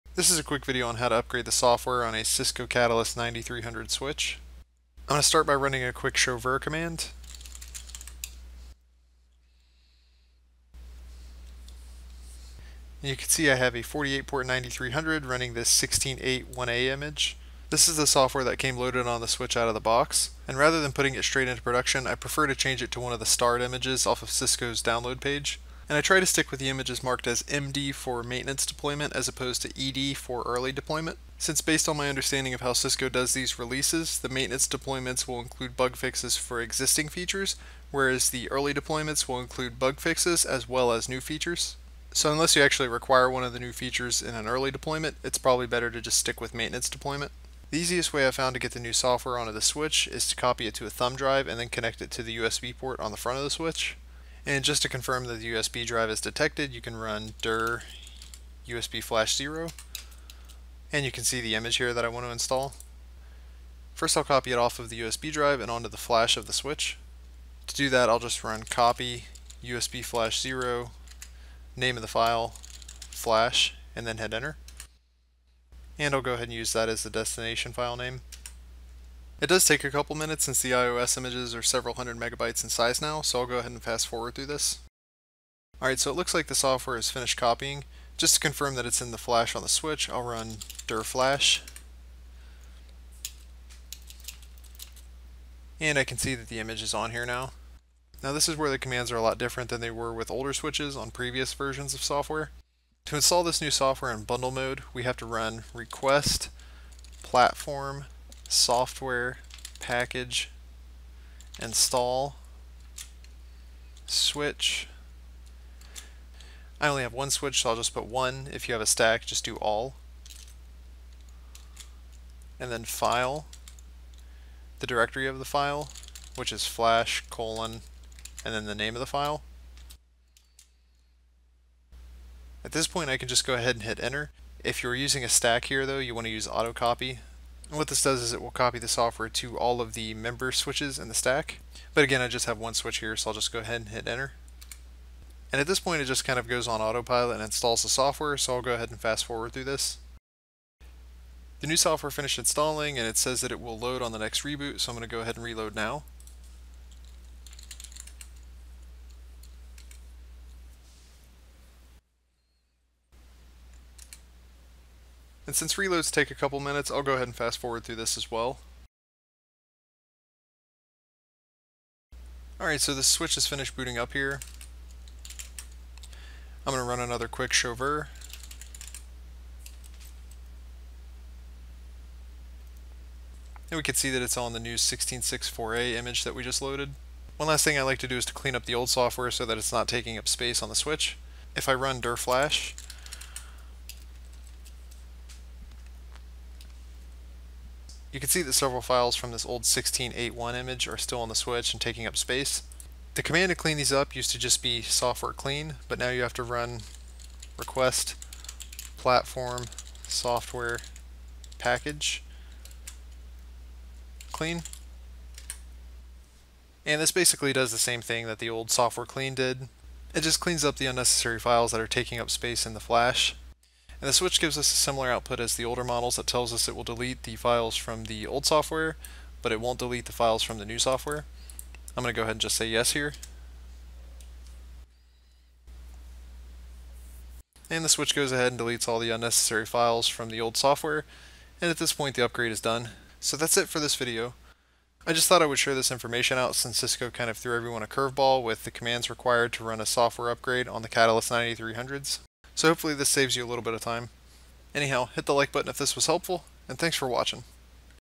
Cisco 9300 IOS Software Upgrade
This is a quick video on how to upgrade the IOS on a Cisco Catalyst 9300.

If you get this error message when attempting to run an upgrade on a stack:
"[2]: FAILED: Specified package file flash:cat9k_iosxe.17.03.05.SPA.bin does not exist"
this can be caused by not having 'auto-copy' in the command.